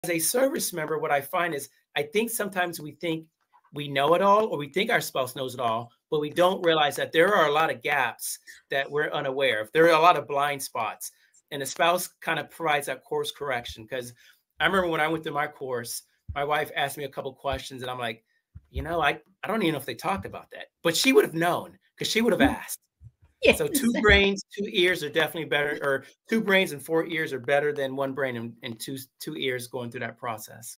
Servicemembers need to include military spouses in the transition process
Include your military spouse in the transition process. Two ears and two brains are better.